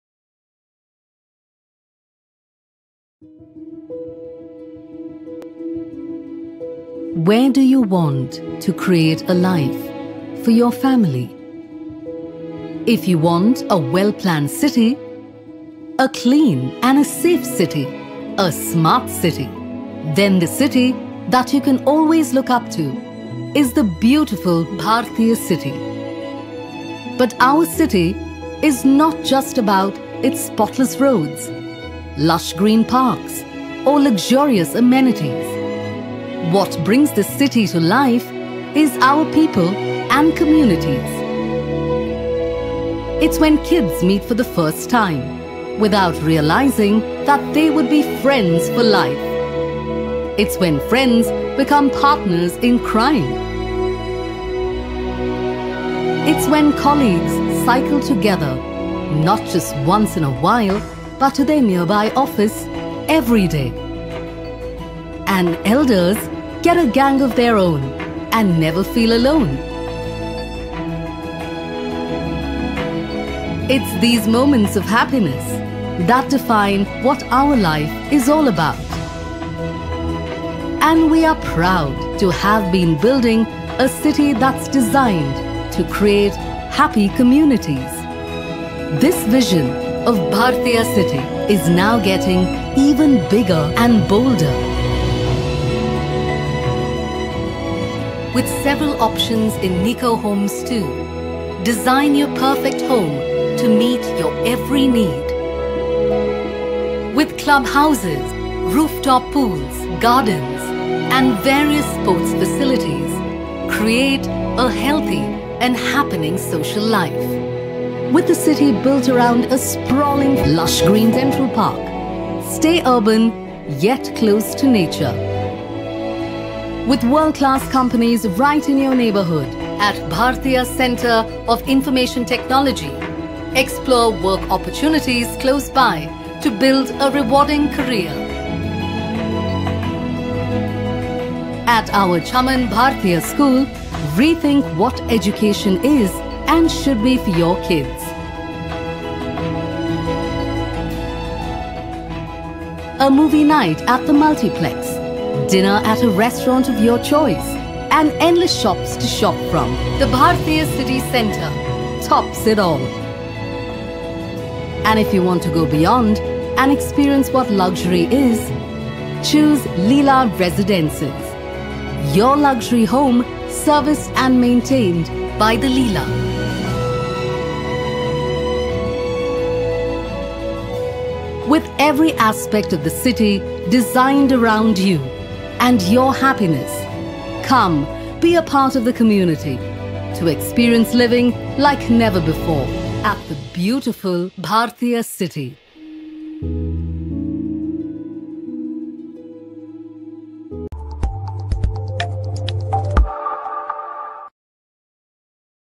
Bhartiya City Nikoo Homes | 𝐏𝐫𝐢𝐜𝐢𝐧𝐠 𝐃𝐞𝐭𝐚𝐢𝐥𝐬
New Phase Coming Soon in 2021

Bhartiya City Nikoo Homes is One of the best planned township in Bangalore. This project is located in Thanisandra, close to city limits. 125 acres of Happiness brings a mix development of Residential Apartments/ Retails & Office Space.

More than a home, it’s a place to call your own. A place where customizable meets convenience and space meets style, but the essence of home is never lost. We’ve got homes for everyone. Homes for the old, homes for the young entrepreneurial spirits and growing families alike. Once you’ve picked the one that’s right for you, everything is customizable in more ways than one.  Just like you, each and every Nikoo home is unique.

Nikoo homes are made-to-measure. Intelligently designed and customizable in more ways than one- we’ll help you to create a home that’s as unique as you are. Quality of life is a shared vision here.

Bangalore mean business, literally. It’s become one of the fastest growing hubs for IT development in the past 8 years. Ever intuitive, our team of master planners had an inkling that we’d go from blossoming to booming, so we built BCIT- a thriving hub for the IT, financial and knowledge driven sectors. It’s home to a dedicated SEZ that follows the utmost security and infrastructure standards, with a prime location to match. The international airport is only 25 minutes away and if you live in a Nikoo home, it’s just a 5 minute stroll – this is the place to do business in Bangalore.

𝗛𝗼𝘄 𝗧𝗼 𝗙𝗶𝗻𝗱 𝗧𝗵𝗶𝘀 𝗩𝗶𝗱𝗲𝗼:
Flats in Bangalore
Nikoo Homes
Bangalore properties
Luxurious Apartments